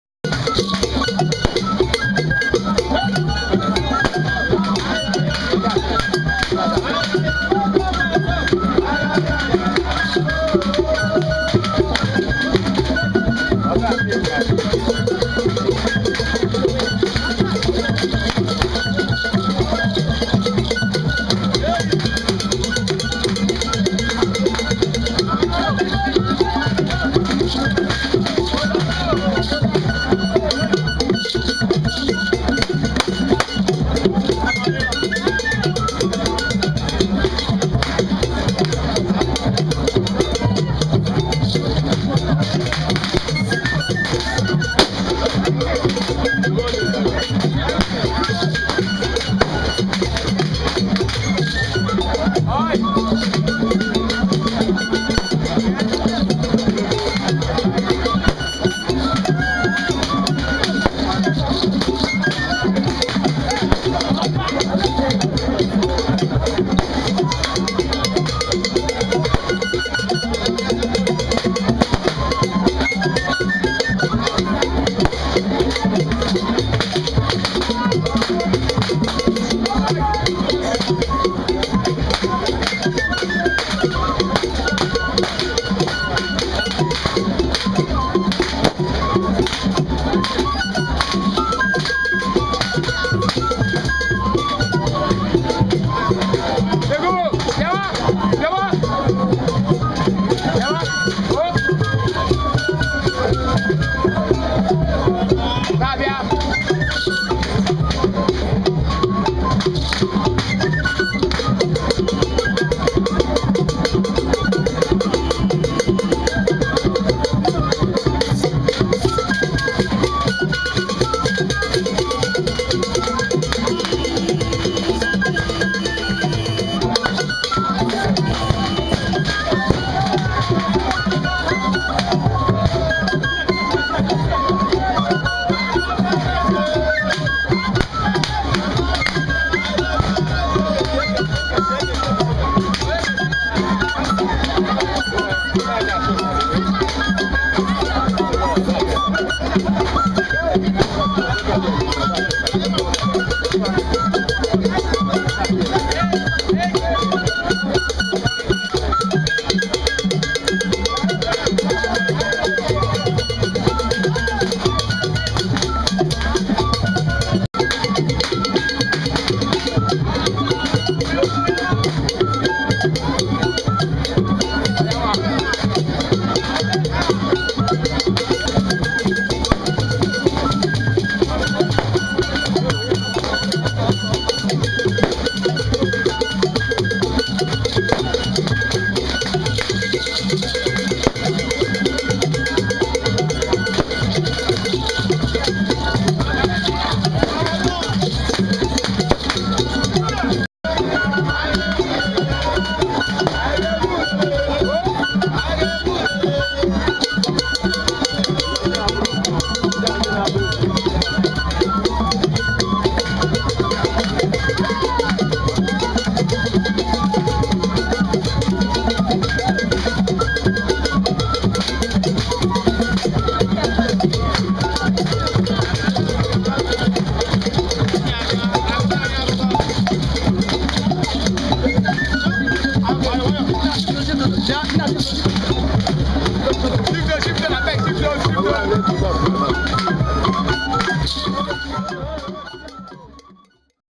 Masquarade dance at Anara Imo State December 26
Anara people of Imo State doing their annual December 26 masquerade dance. Clap for the flutist as you hear the firecrackers exploding all over.